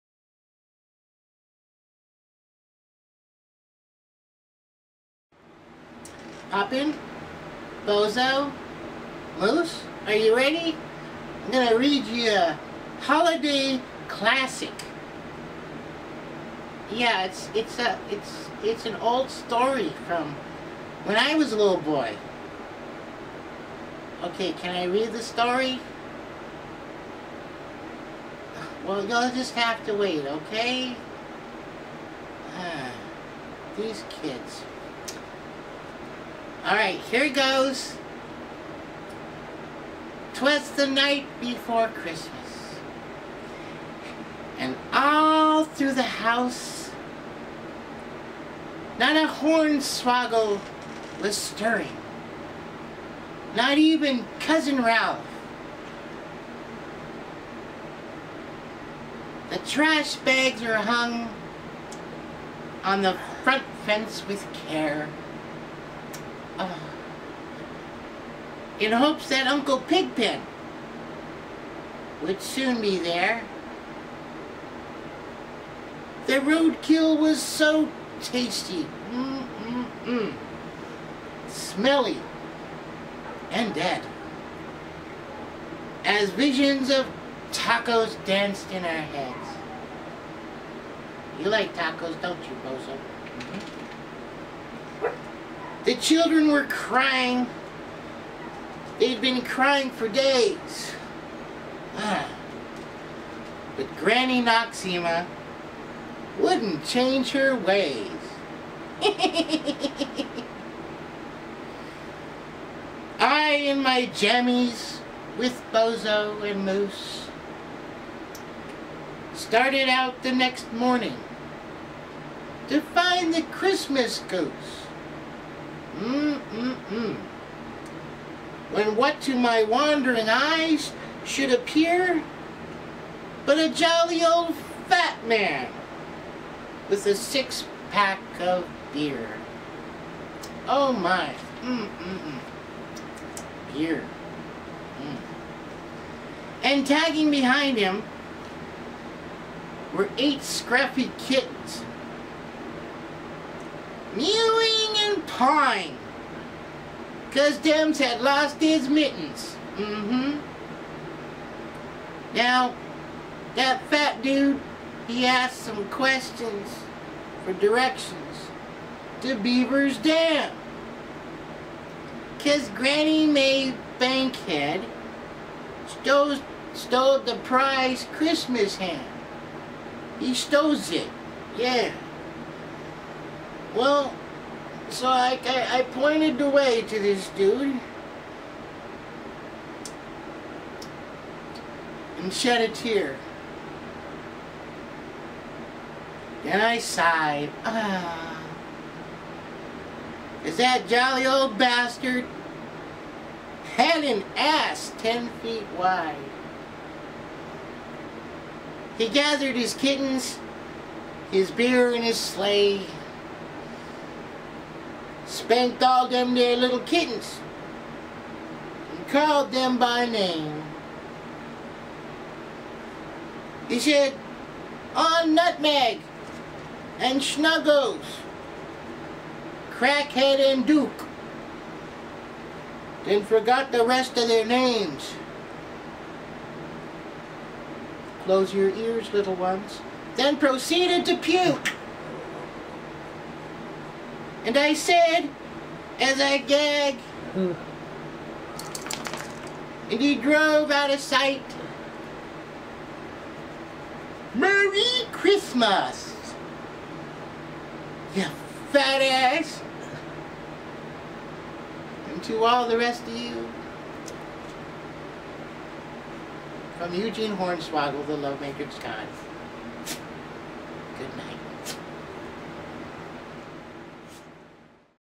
Countdown to Christmas Day 10
A Hornswaggled version of the night before Christmas...Merry Christmas to all and to all a goodnight.